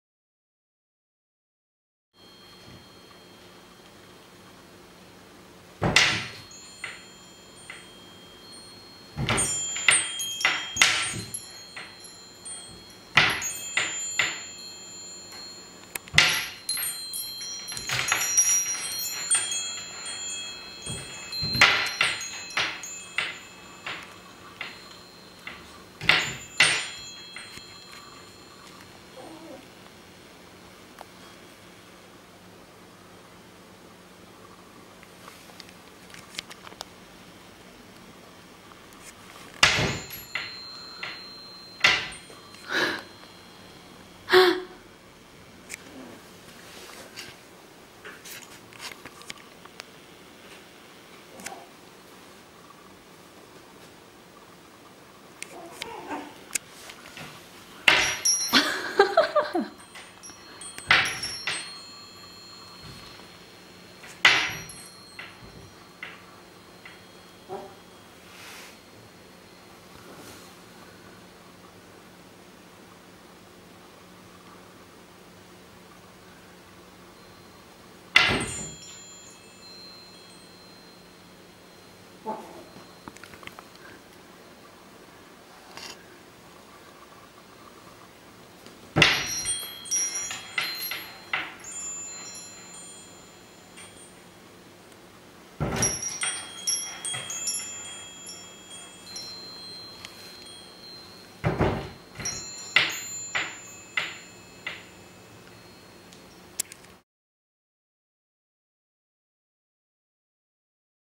Ernie plays bell - British short hair blue kitten
Earnie - 7 weeks old british short hair kitten is playing wind bell.